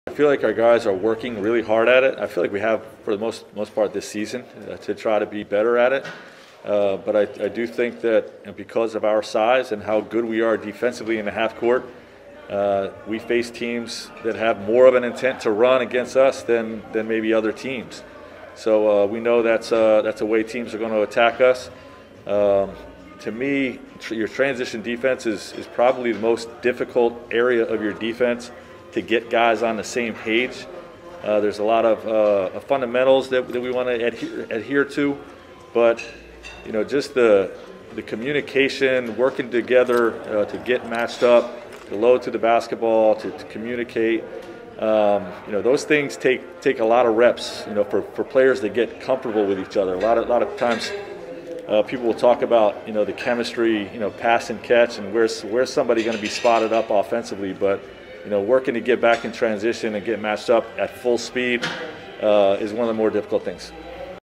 Frank Vogel talks Lakers transition defense
Frank Vogel on the team’s transition defense courtesy of the Lakers.